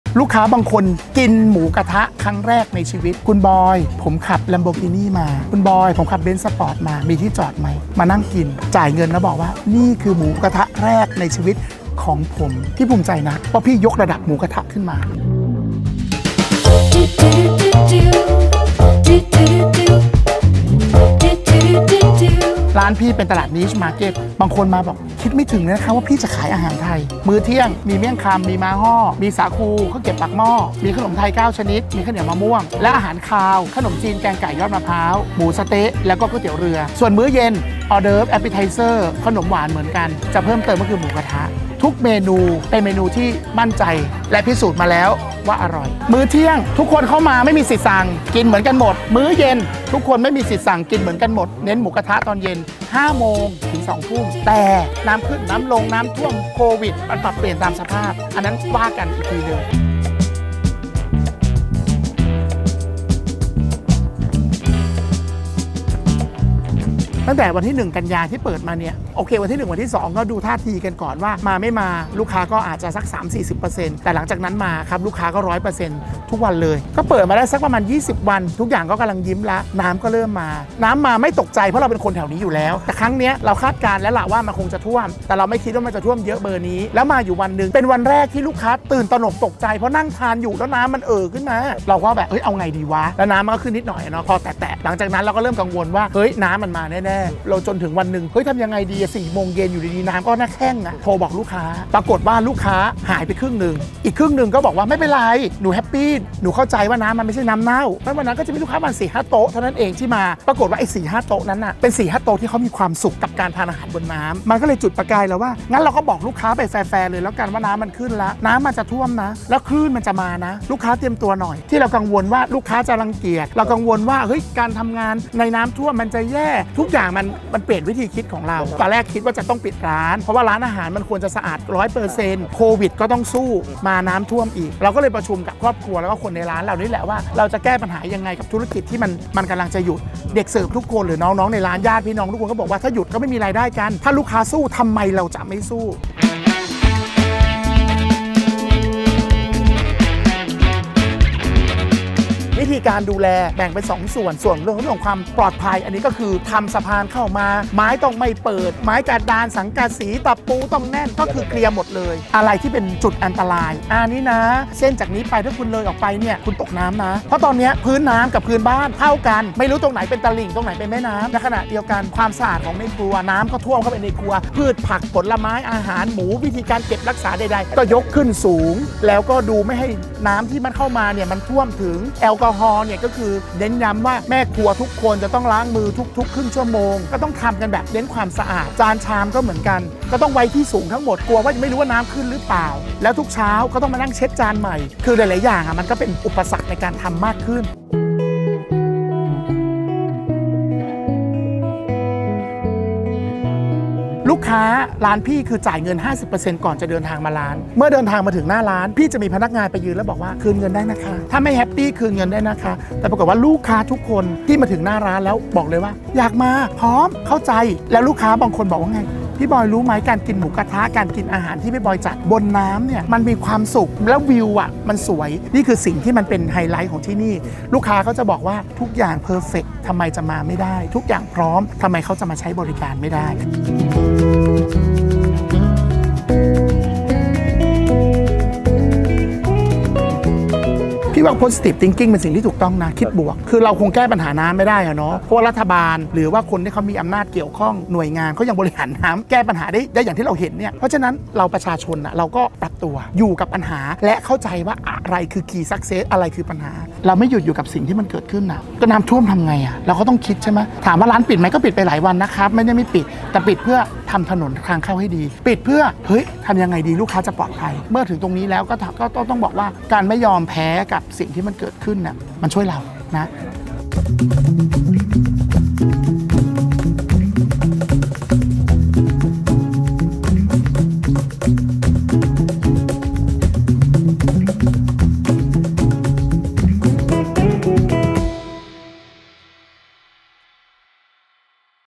หมูกระทะโต้คลื่น
.ยามขึ้นอย่าหลง ยามลงอย่าท้อ เกิดเป็น “คนลุ่มเจ้าพระยา” จะผงาดได้ก็ดั่งใจนึก

.“บอย-ธิติพร จุติมานนท์” เจ้าของร้านบ้านท่าน้ำนนท์ (Chaopraya antique Cafe) ร้านอาหารที่พลิกวิกฤตน้ำท่วม แม้มวลน้ำโถมกระหน่ำซัดถล่มร้านจนหลายๆ ที่ปิดตัว แต่ที่นี้เปลี่ยนให้กลายเป็นความอะเมสซิ่ง!!! ฟินกับการกินอาหารหมูกระทะท่ามกลางสายน้ำและคลื่นลม

.จนกระทั่งดังขจรไกลสื่อสำนักใหญ่ๆ ระดับโลกบินตรงมาทำข่าวแนวคิดและการปรับตัวให้เข้ากับสถานการณ์อย่างมีความสุขและสนุกสนานแบบสามารถทำเงินและสร้างรายได้เยี่ยม กลายเป็นความภาคภูมิใจของคนนนทบุรีพี่น้องพ้องเพื่อนชาวริมน้ำเจ้าพระยา

.3 สิ่งเท่านั้นที่ทำให้เกิดขึ้นส่งให้อาคารเก่าหลัก 100 ปี กับสภาวะน้ำที่ขึ้น-ลงแบบล้นตลิ่งที่คาดเดาไม่ได้ก่อร่างสร้างเป็นบรรยากาศหลักล้านๆ ในราคาหลักร้อยแบบสบายๆ คลูๆ ไม่เสียงบแก้ปัญหาให้วุ่นความ

1.เพียงเพราะหัวใจรักในการคิดบวก
2.เพียงเพราะหัวจิตไม่ยอมจำนนยอมแพ้
3.เพียงเพราะหัวคิดแต่ปรับตัวให้เข้ากับบริบทที่มีอยู่อย่างเข้าใจ